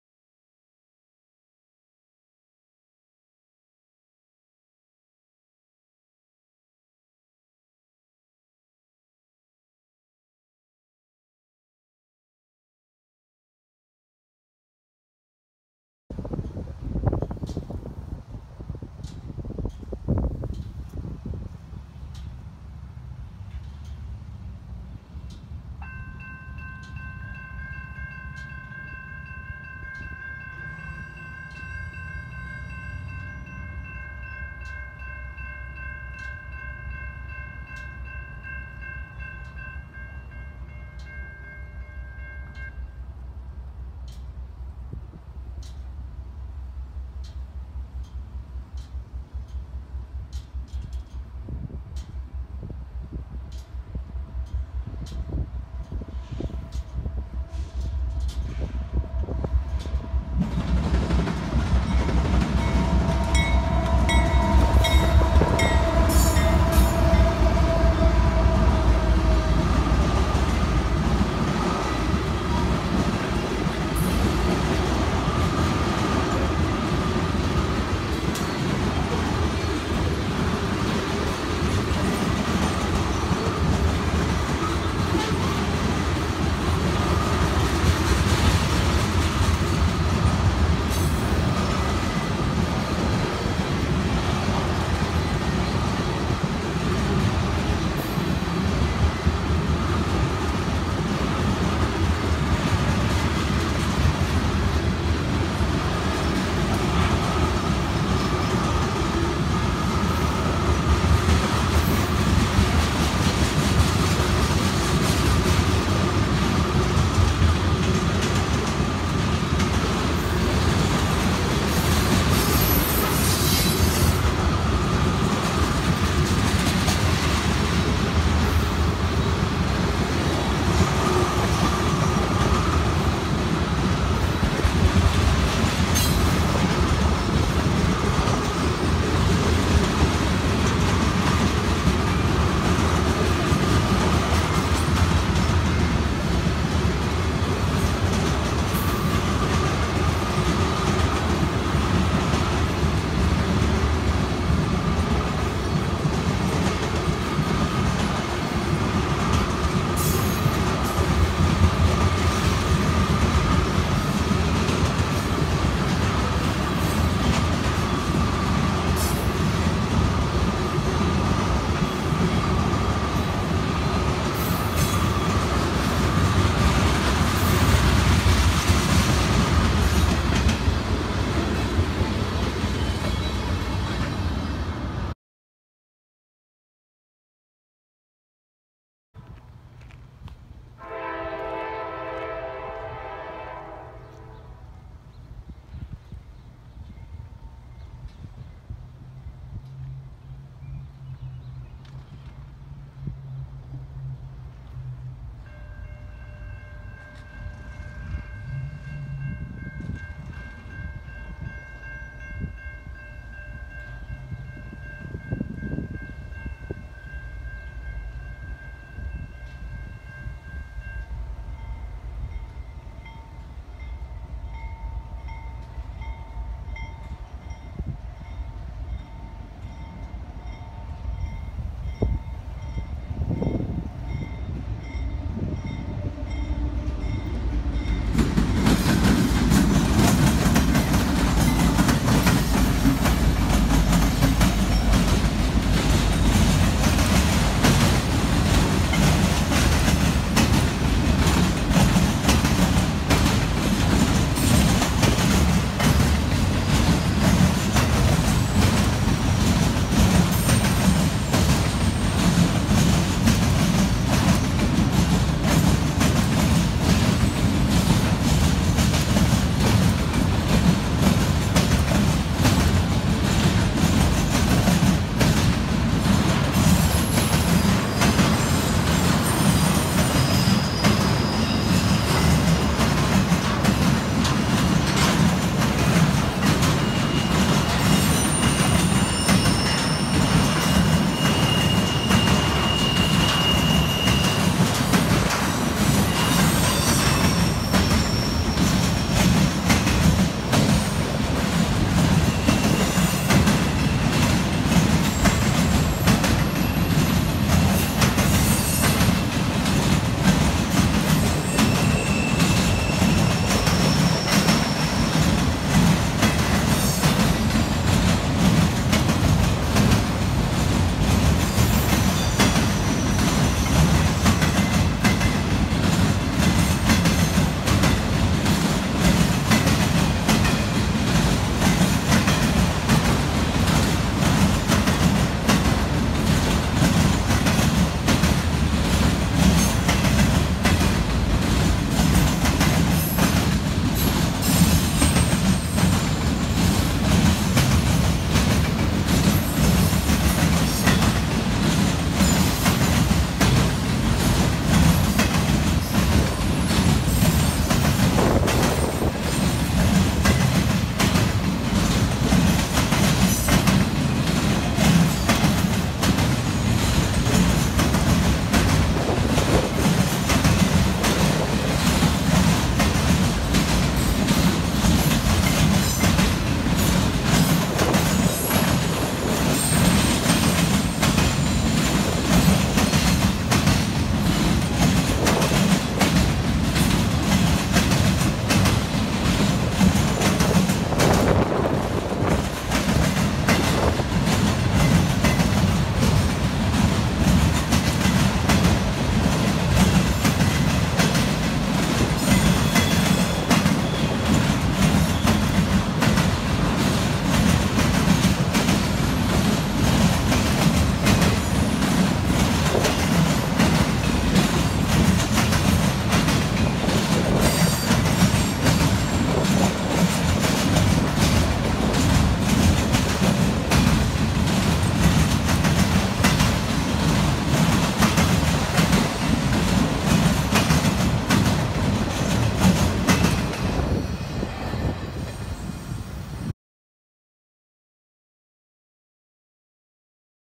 5/14/2017 - Muncie, IN - CSX & NS crossing the diamonds.
Despite a slow start, was able to video two trains crossing the Downton diamonds at Walnut St., CSX CP 229.  First was a westbound CSX oil can and then a northbound Norfolk Southern auto rack on a beautiful sunny day.
It was also nice to meet several young rail fans, including Cody and Alex.   It's good to see that this hobby/interest continues with a young generation!